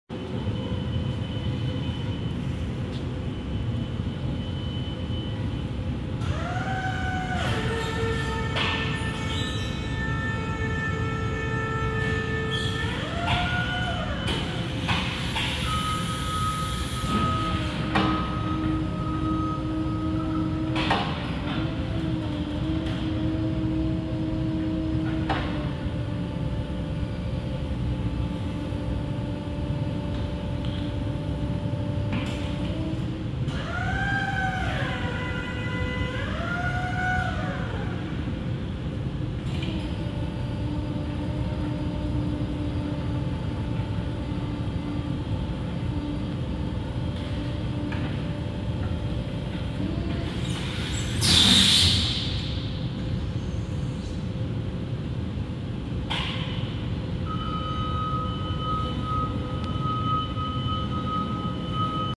Raymond Warehouse Forklift
Raymond Warehouse Forklift, Mdl: SA-CSR 30T, 48V Battery, 3,000 lb Cap, 240" Lft Ht, Swing Forks 41"

This Lift will be offered at auction on 10/9/24 . Please see the link below to bid on this item.

HAVE EQUIPMENT TO SELL?